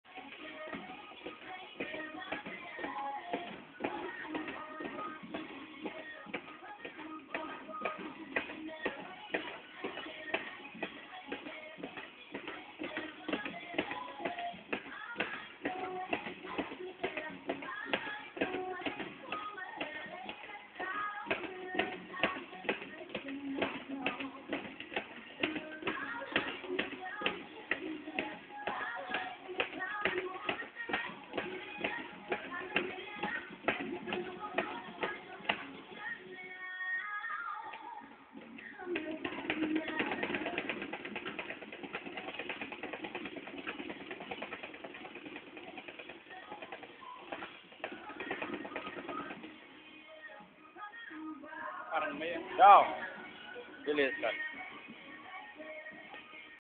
LINCOLN BALA PUNCHING BALL.3gp
Lincoln bala praticante de boxe e muay thai dando inicio no treinamento de punching ball na academia completa